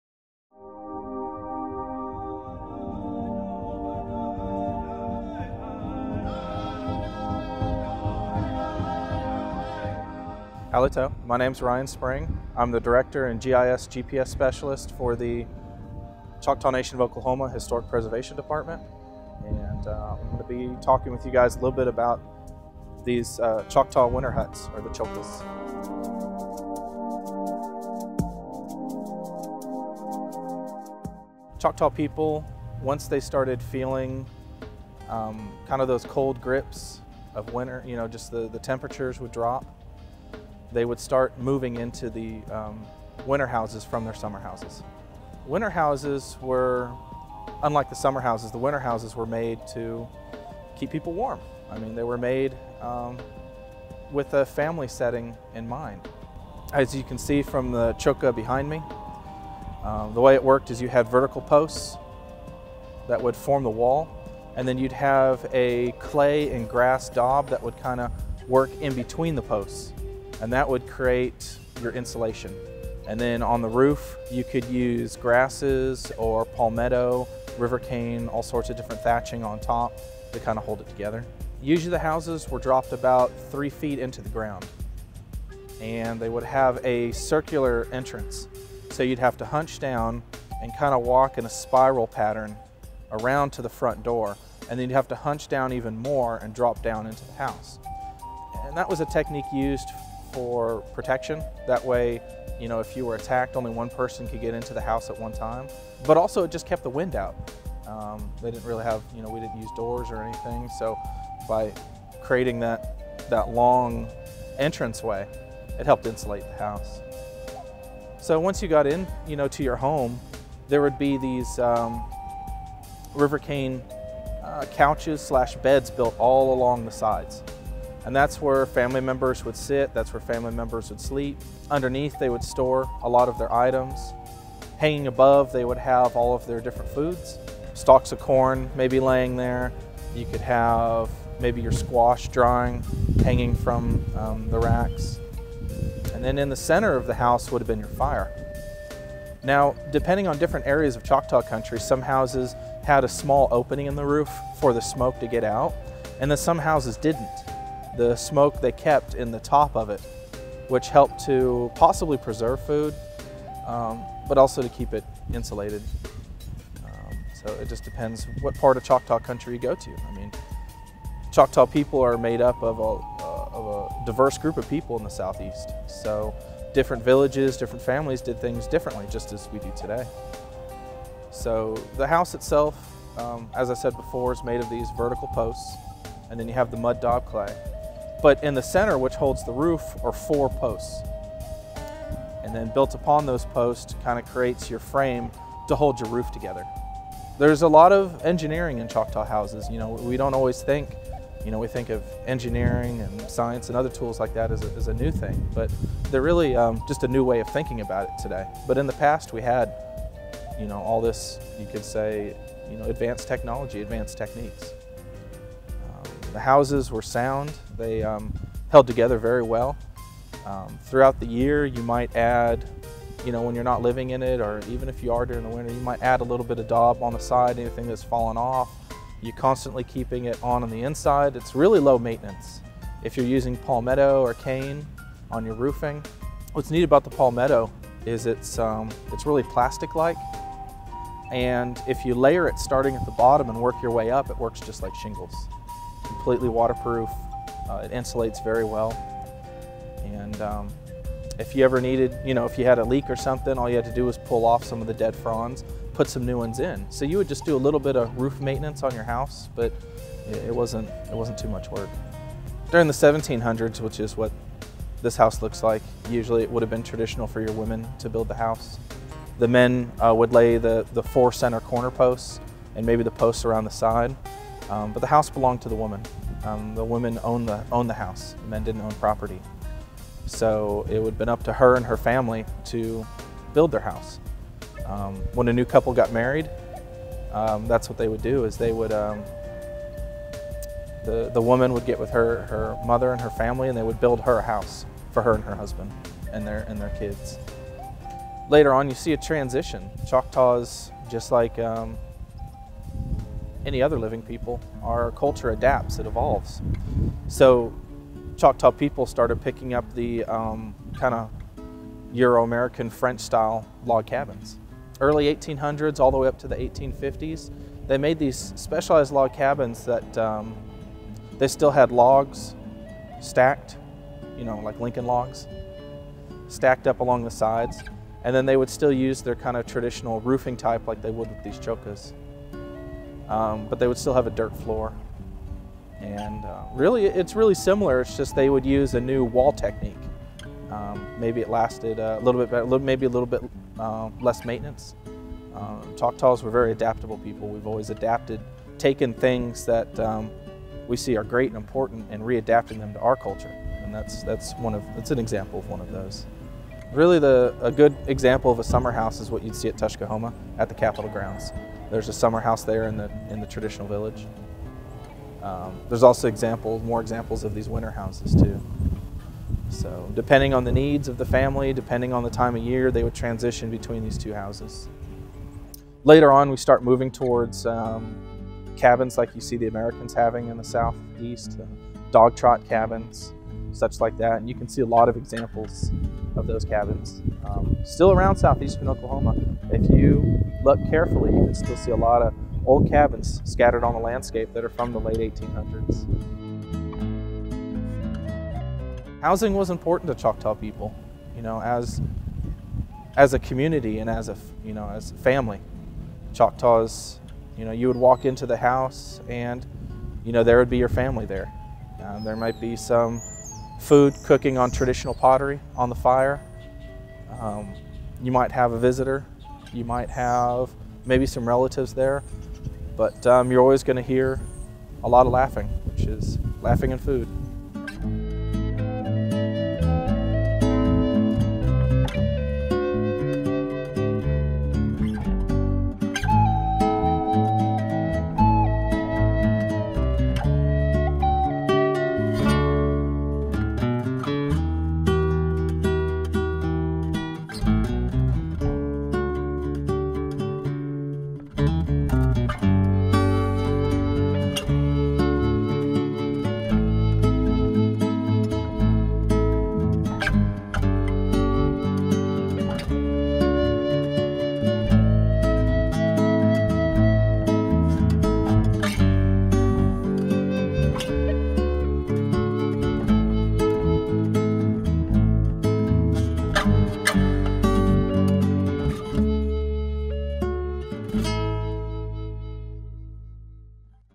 Chukka
The Chukka or Choctaw traditional house/ hut was vital in the survival of the Choctaw people.  Ryan Spring of the Choctaw Nation Historic Preservation Department explains why the Chukka was so vital to the Choctaw and how they were constructed.